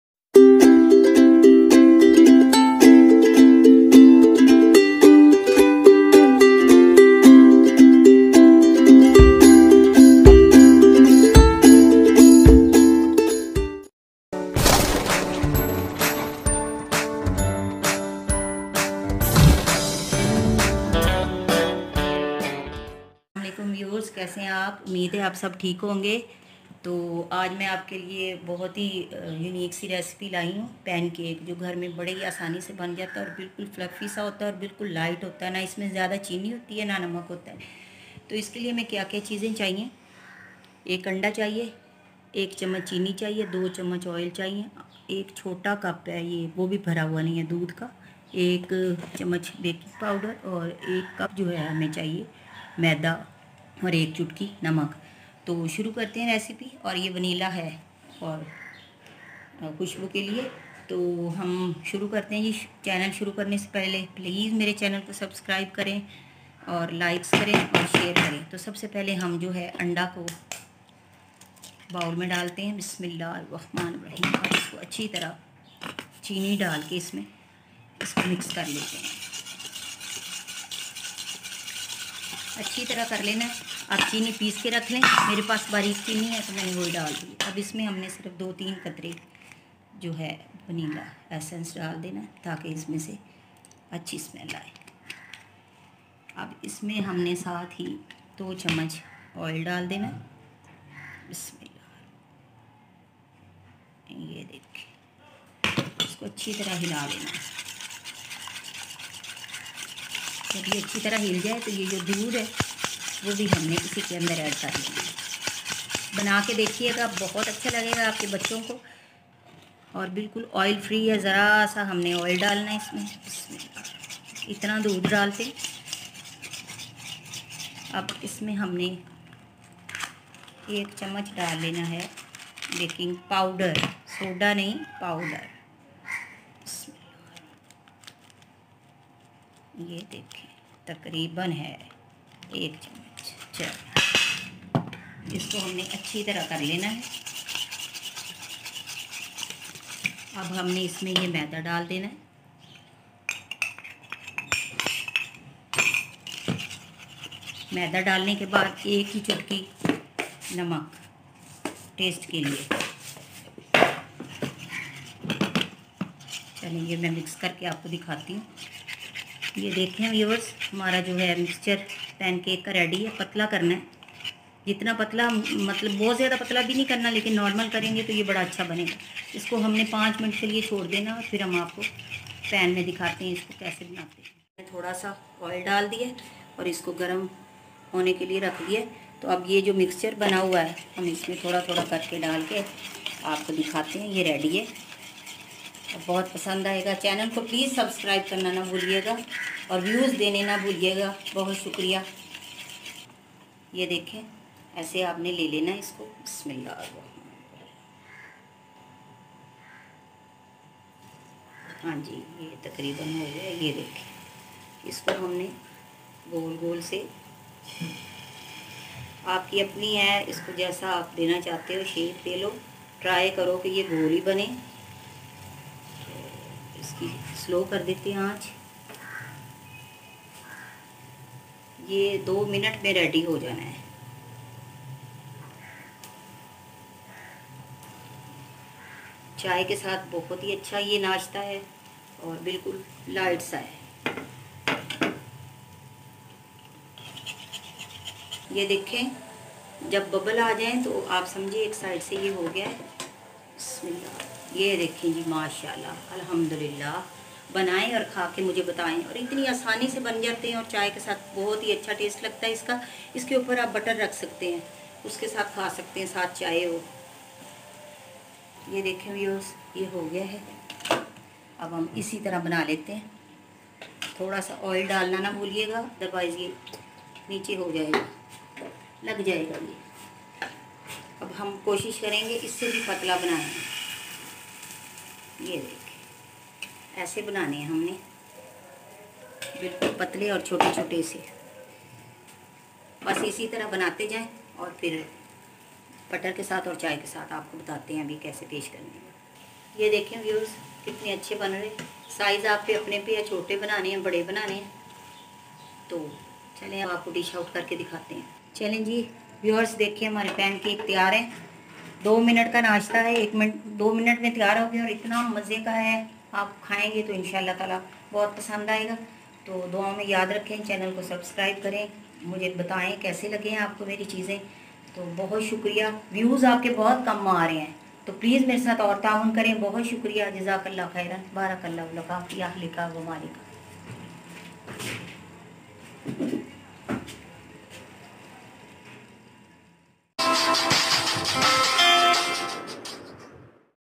Breakfast Special Pancakes recipe in few minutes| Healthy Breakfast|khana with Rukhsana
In this video, I will show you how to make spongy and delicious Pancakes for breakfast in just 2 minutes.